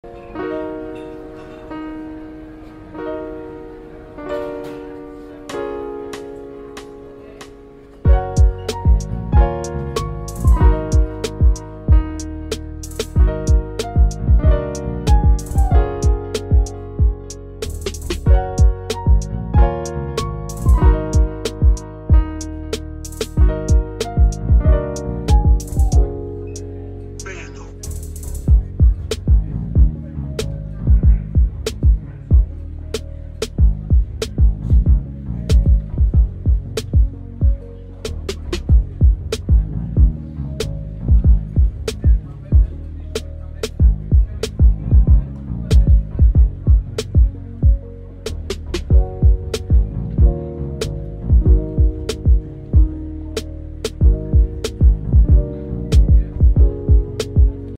Indian Gharial San Diego Zoo Compilation with Lofi
Indian Gharials at San Diego Zoo compilation with Lofi